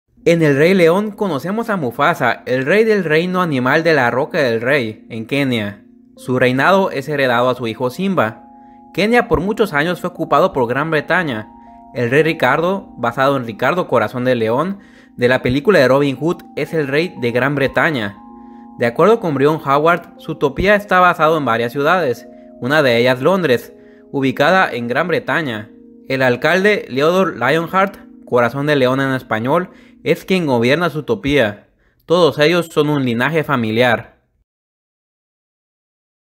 Mufasa y Simba del REY LEON son ancestros del Rey Ricardo y Leodore de ZOOTOPIA
¡Hola a todos! El día de hoy les traigo esta teoría de Disney de las famosas películas como el Rey León, Zootopia y Robin Hood. En la cual unimos a los Reyes Mufasa, Simba, Ricardo Corazón de León y el Gobernador Leónzales, en esta caso en ingles, Leodore Lionheart.